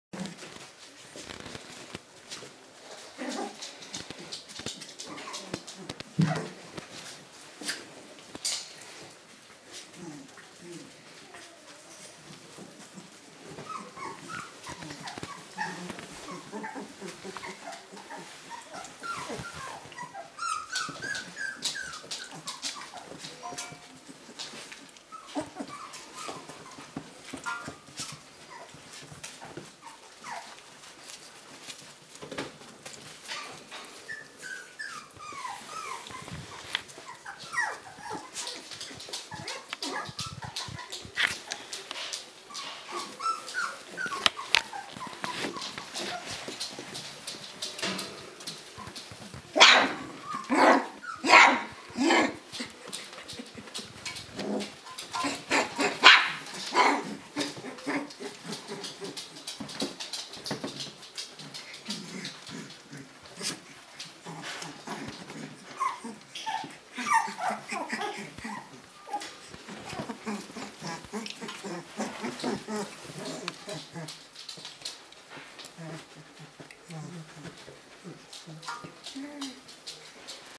R 4Weeks 4
Štěňátka vrh R, 4 týdny stará. Hraní si v ohrádce